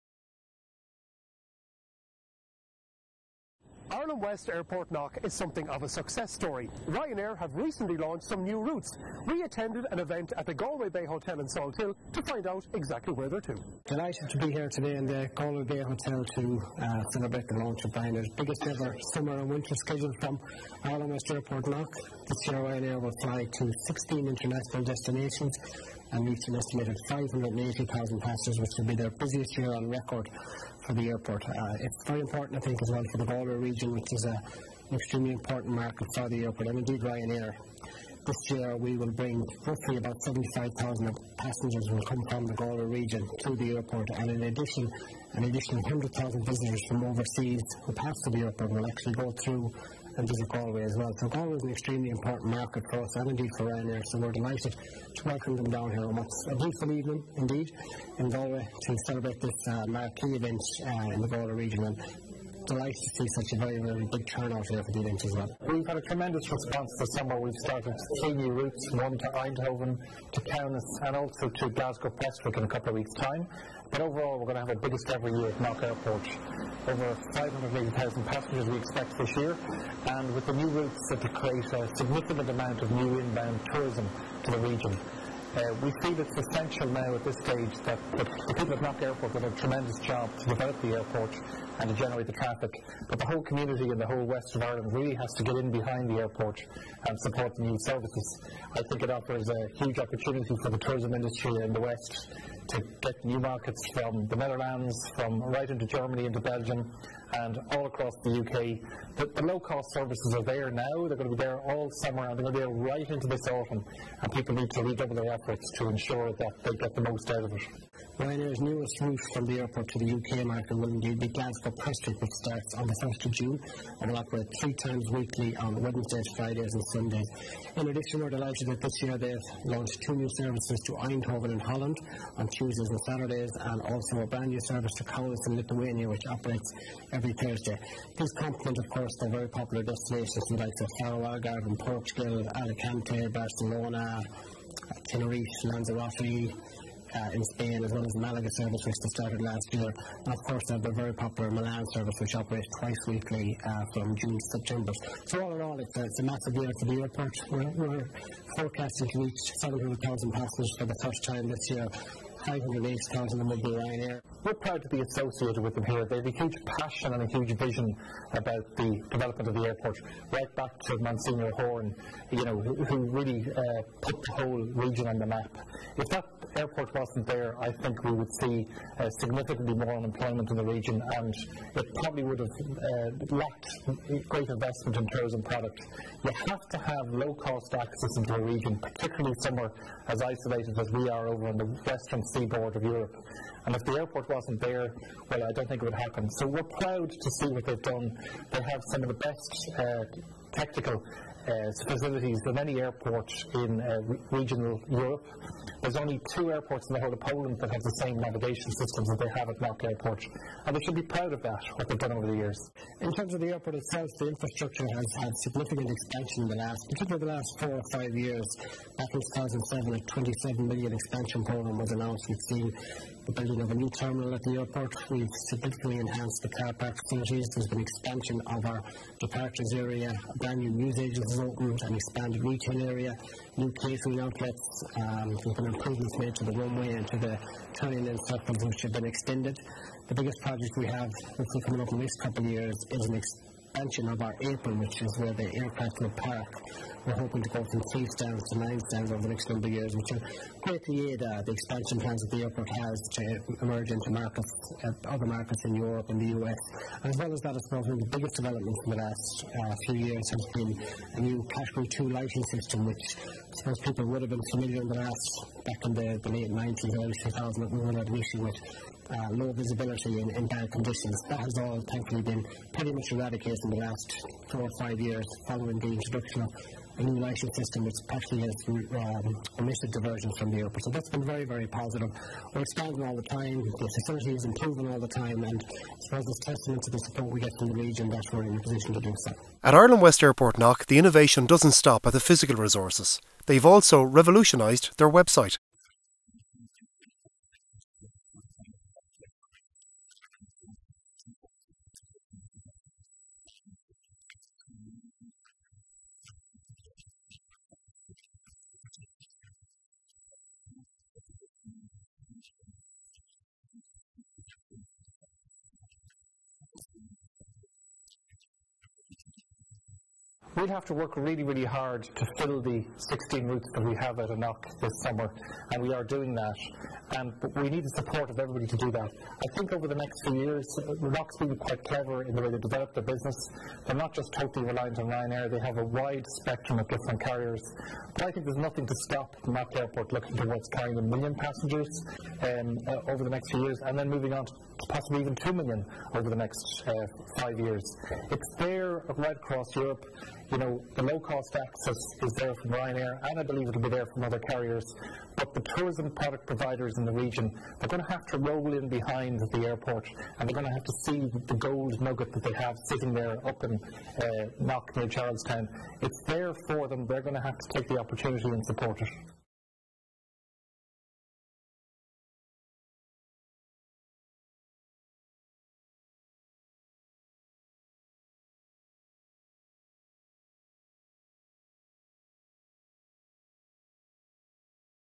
Ryanair summer launch piece May 2014
Launch of Ryanairs biggest ever summer and winter schedules from Ireland West Airport Knock which took place at the Galway Bay Hotel on May 15th 2014